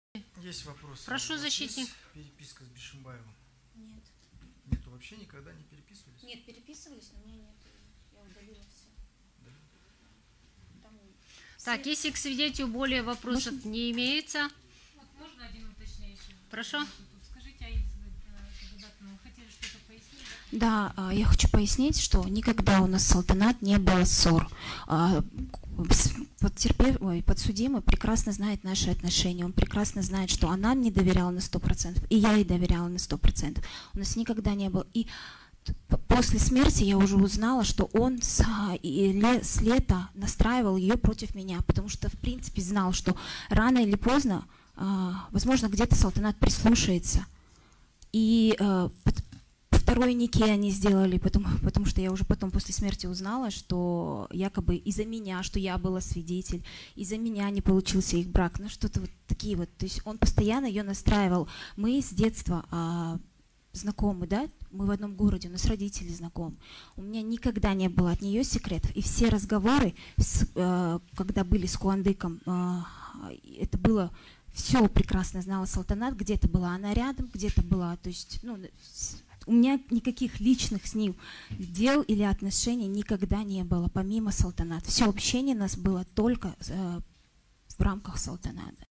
"Он настраивал ее против меня": подруга Нукеновой о Бишимбаеве
Подруга Нукеновой на судебном заседании рассказала, что Куандык Бишимбаев пытался настроить Салтанат против нее
Актуальные новости текущего часа в Казахстане.